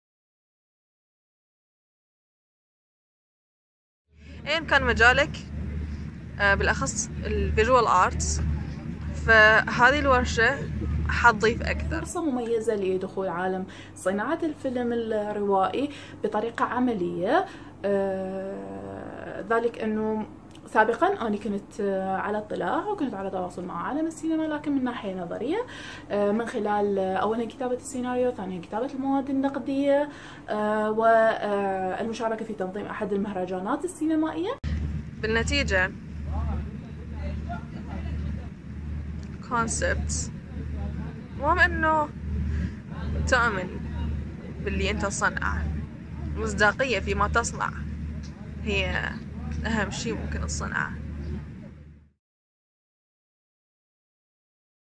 Women Make Film Participants' Statement
In this video, we are taking you back to the project with statements from two of our participants Zahraa and Kawthar who told us what the project meant for them.